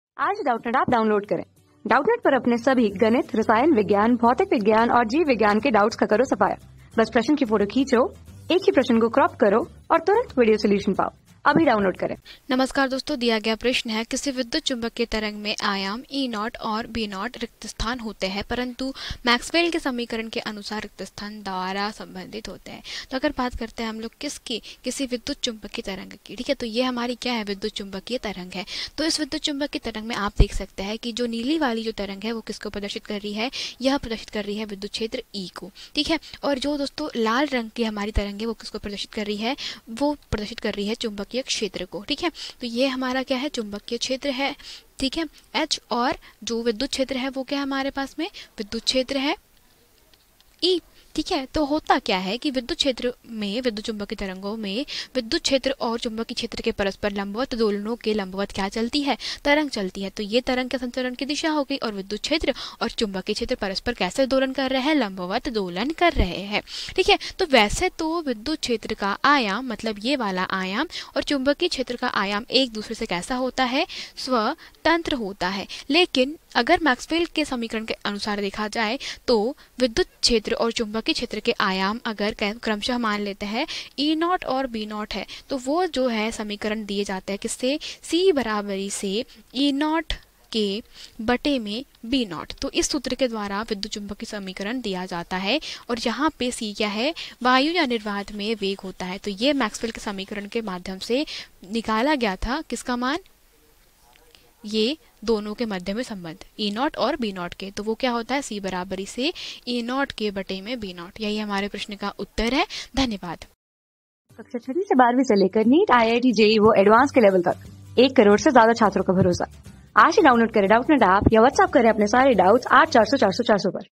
किसी वैद्युतचुम्बकीय तरंग में आयाम E_0 और B_0 ___ होते हैं परन्तु मैक्सवेल के समीकरण के अनुसार _...
किसी वैद्युतचुम्बकीय तरंग में आयाम E_0 और B_0 ___ होते हैं परन्तु मैक्सवेल के समीकरण के अनुसार ___ द्वारा सम्बन्धित होते हैं।
Class: 12
Subject: PHYSICS
Chapter: वैद्युतचुम्बकीय तरंगे
Board:IIT JEE

👉 Have Any Query? Ask Us.